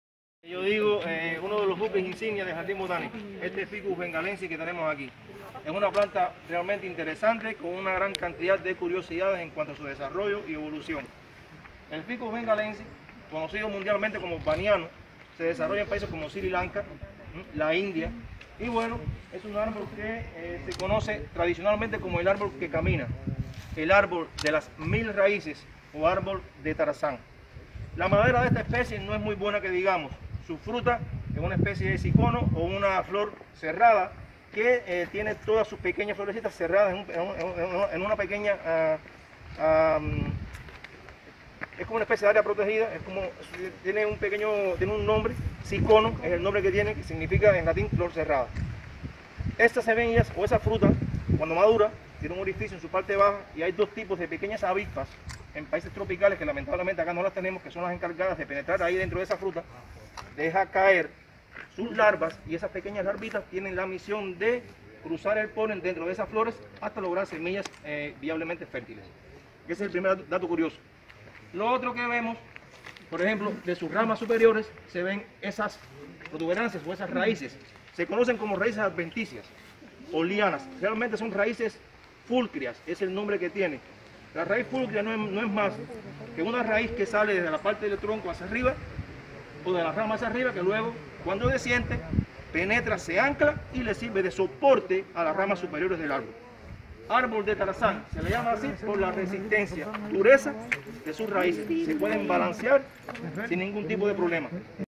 El árbol que camina en el Jardín Botánico de Cienfuegos
El baniano (Ficus benghalensis), nombre común que comparte con otras especies del género Ficus, es un árbol endémico de Bangladés, India y Sri Lanka, conocido popularmente en el Jardín Botánico de Cienfuegos como El árbol que camina. El instructor-guía Roger Echeverría brinda algunos datos de esta especie.